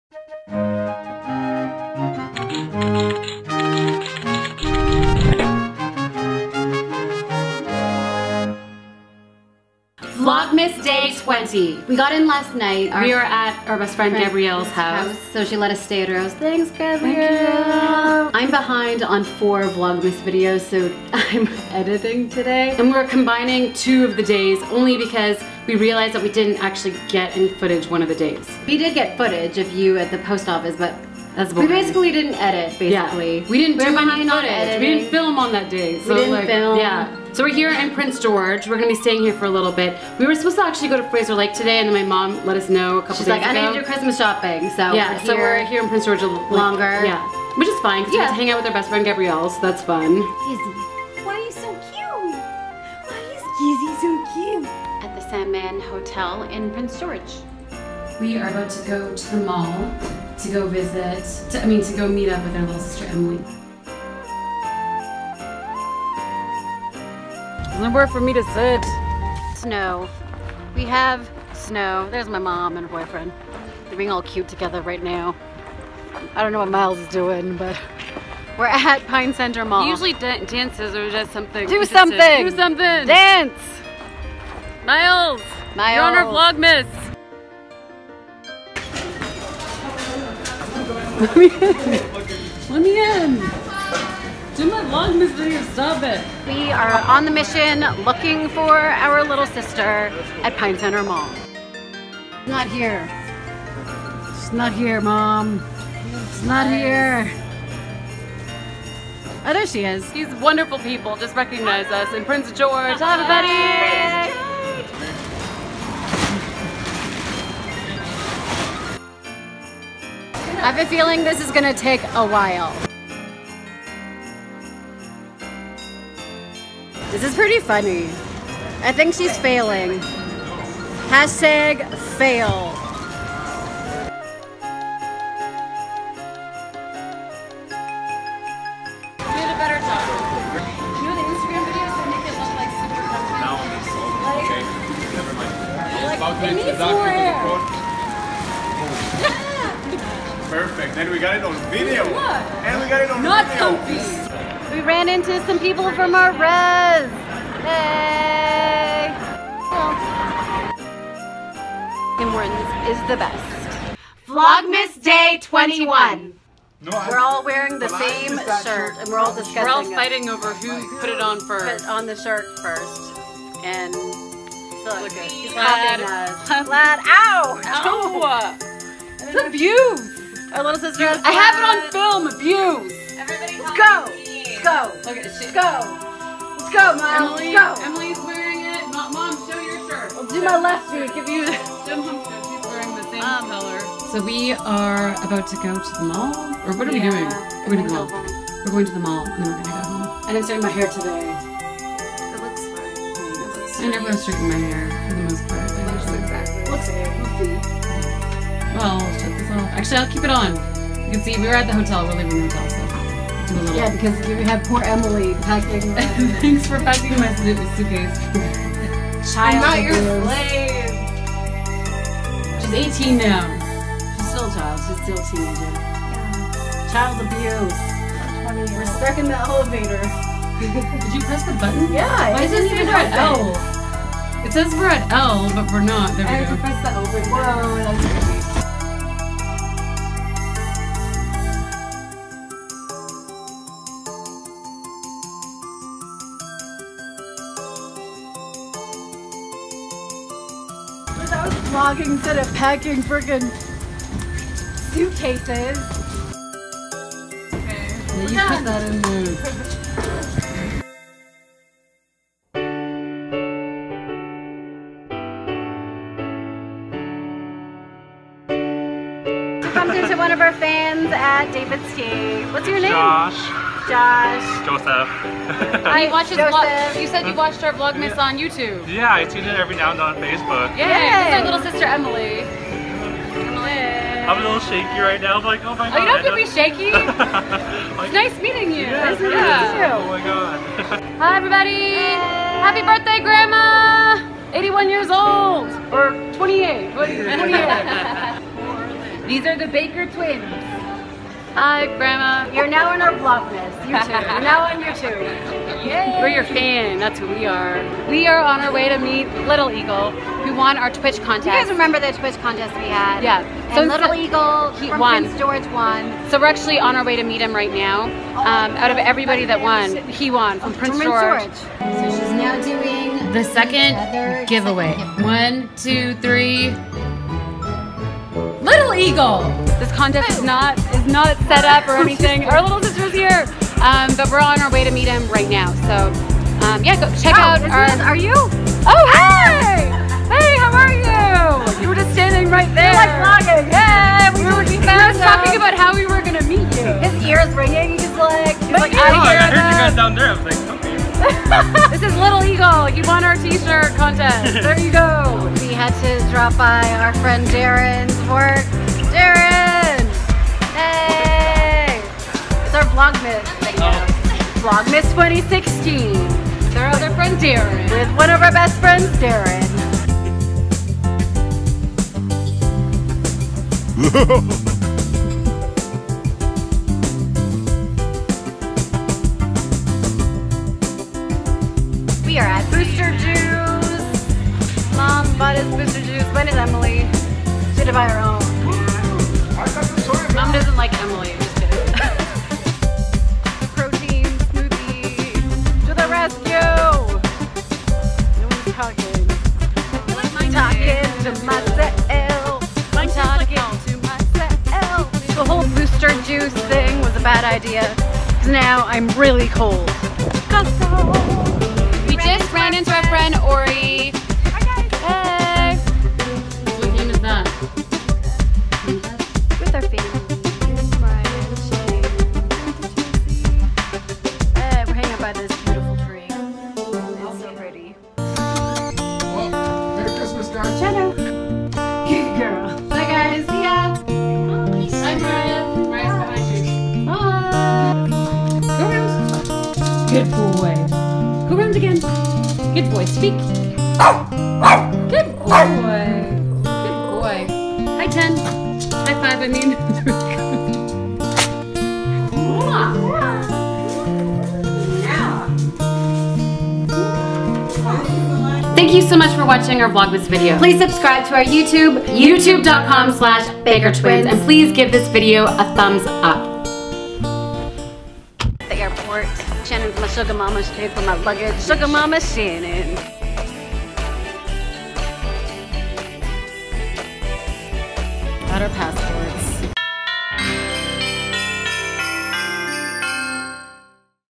Meeting fans at the Mall! | Vlogmas 2016 | Day 20 & 21 | Baker Twins
Errands & shopping in Prince George! Plus we got to meet a ton of fans at the mall!

We also have SnapChat!!

Send us Letters!!!